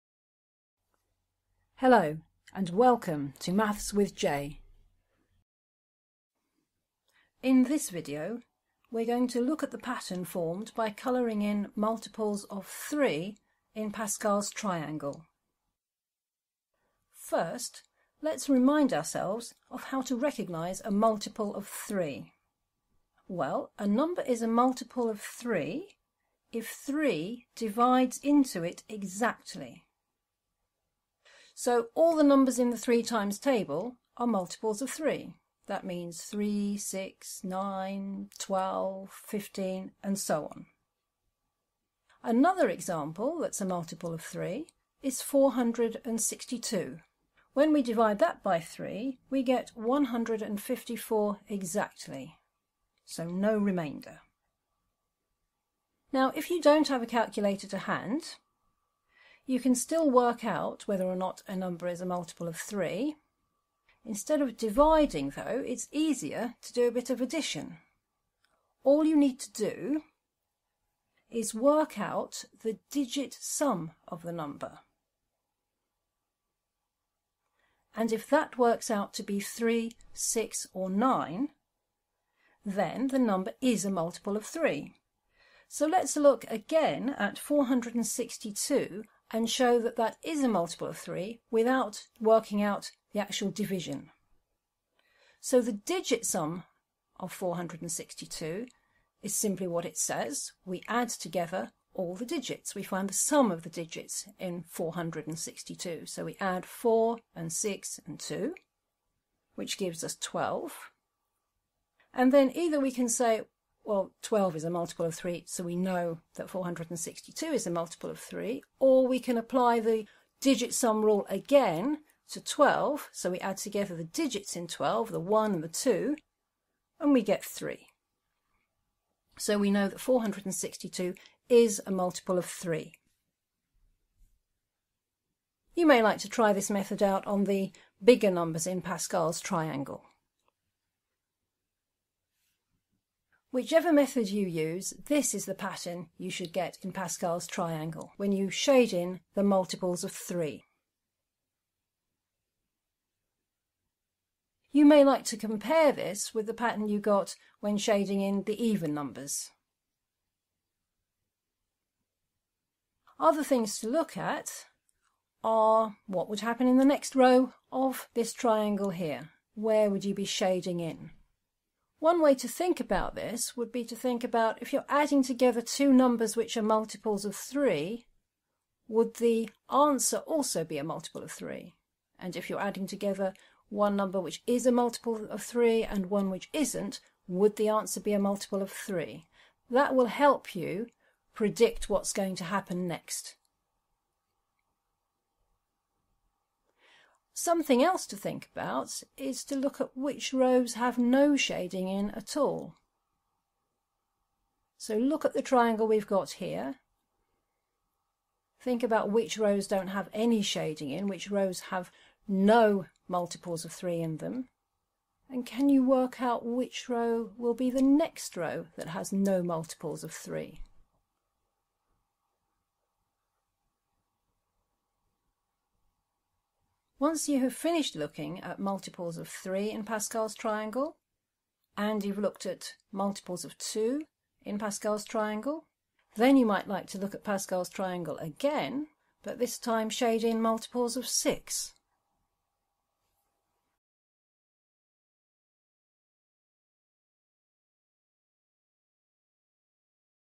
Multiples of 3 in Pascal's Triangle
Colouring in multiples of 3 in Pascal's Triangle. Can you continue this pattern? How does it repeat?